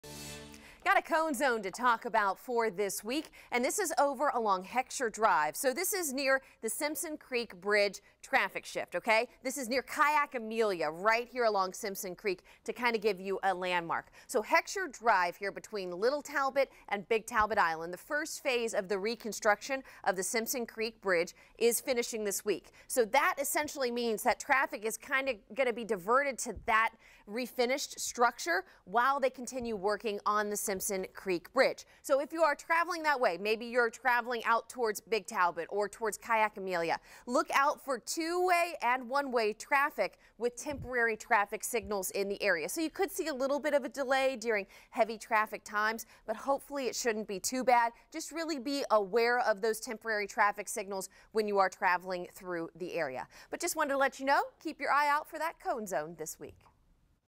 Cone Zone: Heckscher Drive on Little Talbot Island
First phase of reconstruction to finish this week.